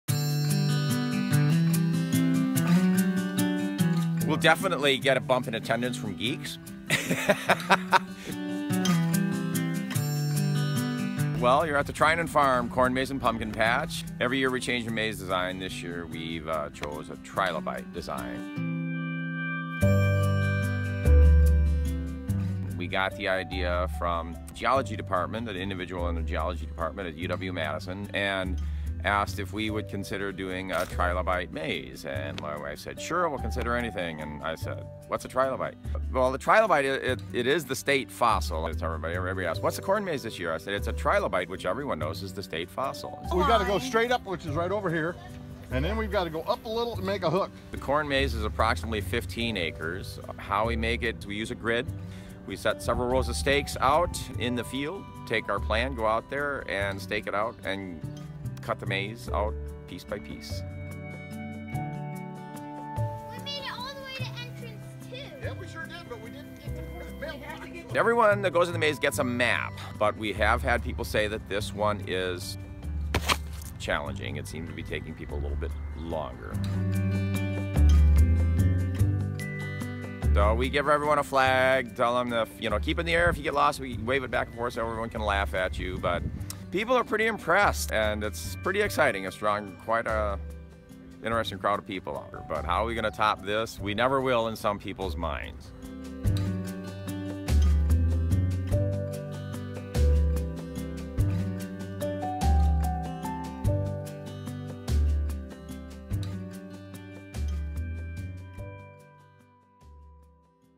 Drone footage of a fossil-shaped corn maze
The Treinen Farm corn maze in Lodi, Wis., features Wisconsin's state fossil, the trilobite. The idea behind the famous award-winning corn maze came from the University of Wisconsin-Madison Geology Museum.